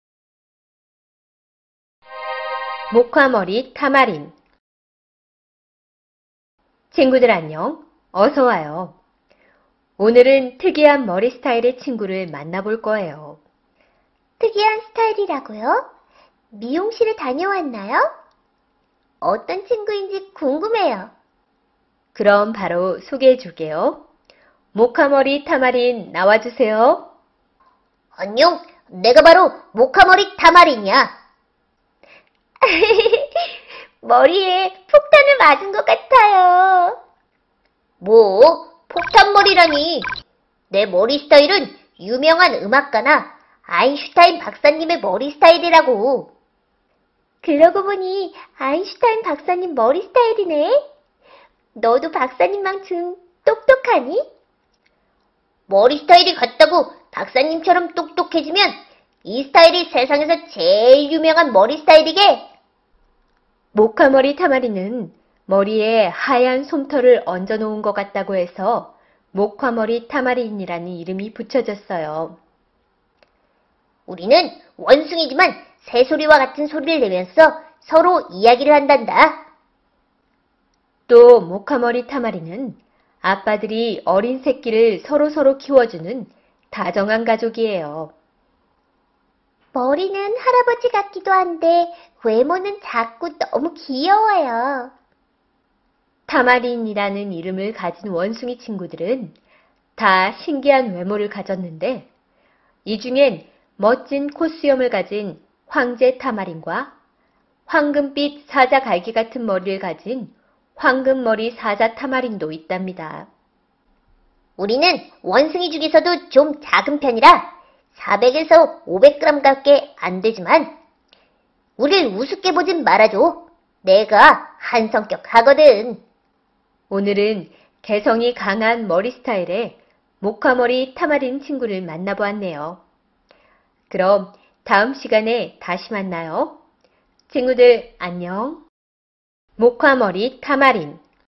목화머리타마린
아이들이 좋아하는 캐릭터가 동물들에 대해 설명해 줍니다.교육에 좋아요.